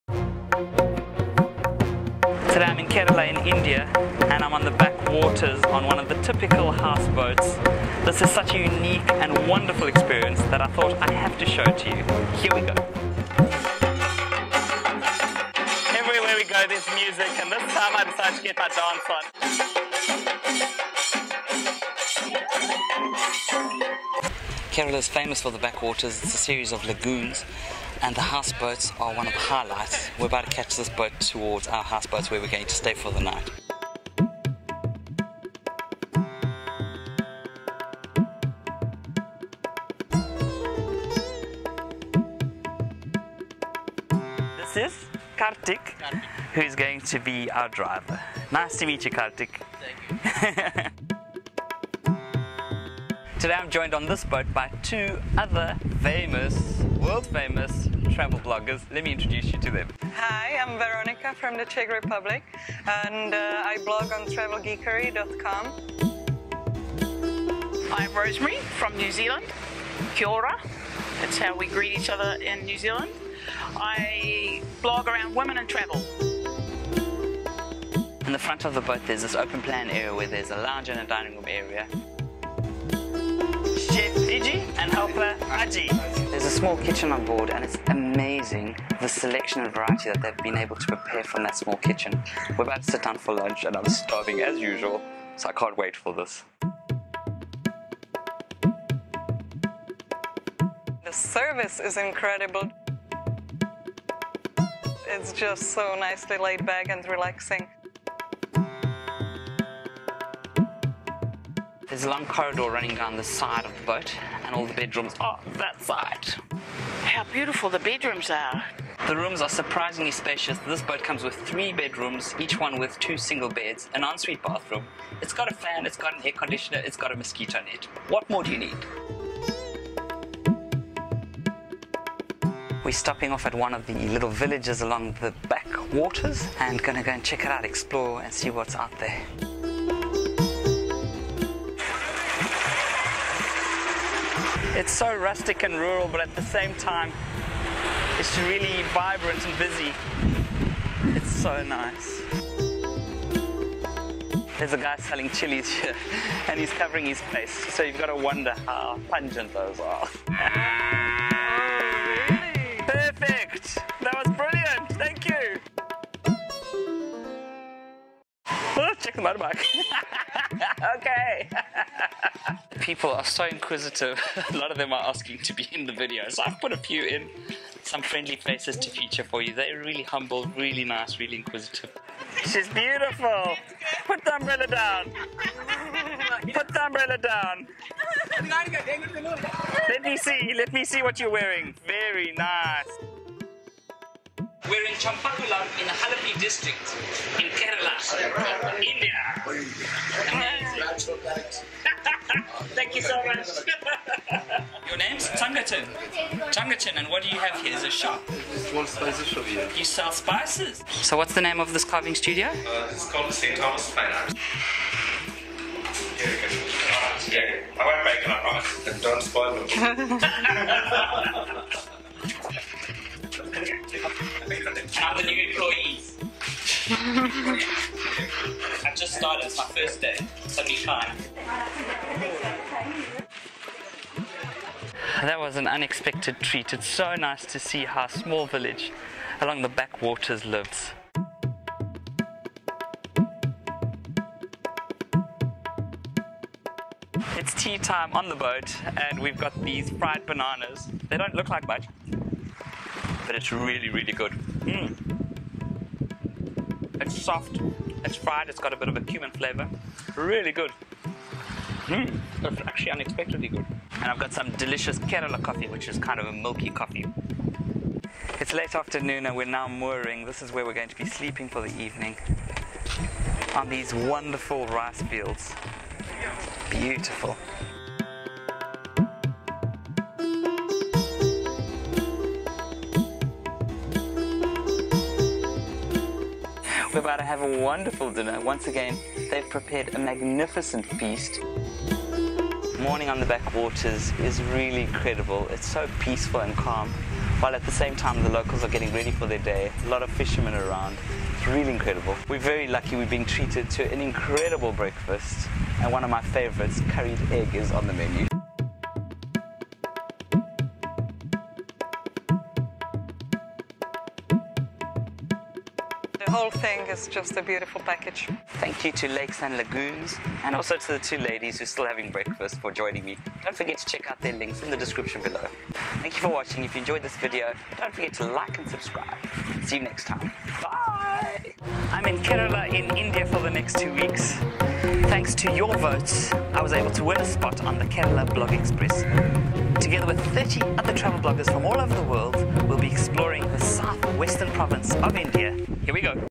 Houseboat in Kerala India Backwaters
The backwaters of Kerala India are a unique and magical place. Experiencing the backwaters in a houseboat or Kettuvalloms is one of the best things to do in Kerala. As part of the Kerala Blog Express 2 (a sponsored trip), I was able to spend an evening on a three roomed house boat and stop off in Champakulam to meet the local population. We caught the houseboat from Alleppey and sailed along the Kerala Backwaters.

A big thanks to Lakes and Lagoons who sponsored this trip.

Music and sound effects are provided by Jinglepunks.com.